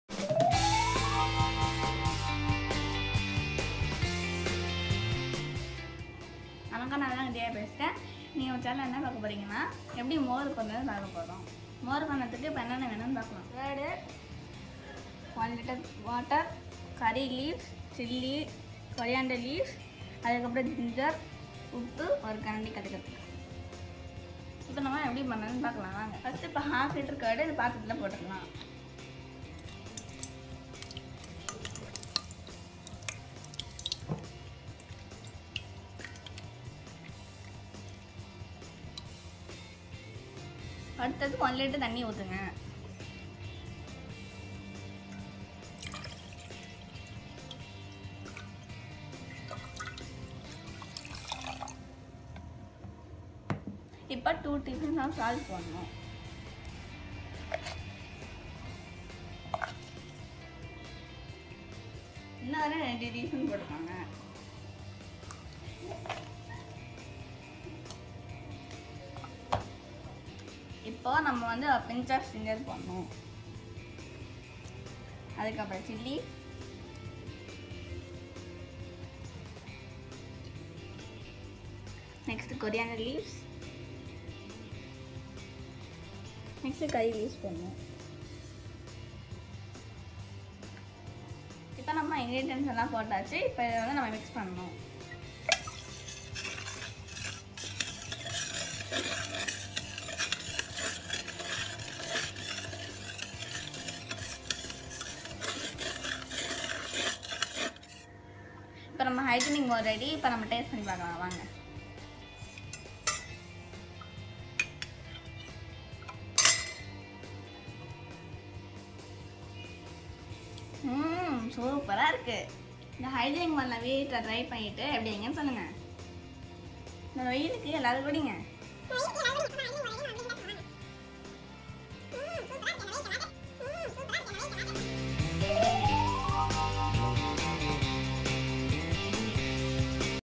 Tanda Moore for Hot Summer | Dhiya Ramakrishnan
In this video, we are covering on how to prepare buttermilk quick and easy with less ingredients. Ingredients required are half liter curd, one liter water, half-inch ginger finely chopped, handful of coriander leaves and curry leaves, two green chilli and salt to taste. Add all the ingredients in a pot and Stir well. Buttermilk is ready to serve. Enjoy drinking buttermilk in this hot summer.

Butter milk (Moore) improves oral health, supports strong bones, helps lower cholesterol and blood pressure levels. It is one of the finest and essential drink to cool our body. Buttemilk is high in potassium, Vitamin B12, Riboflavin, calcium and also a good source of Phosphorous. All in all, it’s a super coolant for hot summer.